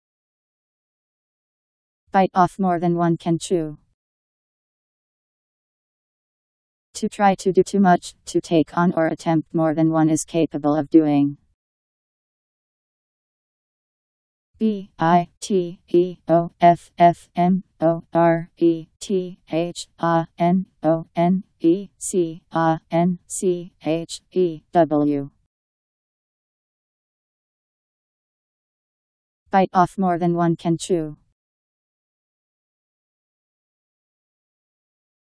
Bite off more than one can chew Meaning
Video shows what bite off more than one can chew means. To try to do too much; to take on or attempt more than one is capable of doing..  Bite off more than one can chew Meaning. How to pronounce, definition audio dictionary. How to say bite off more than one can chew. Powered by MaryTTS, Wiktionary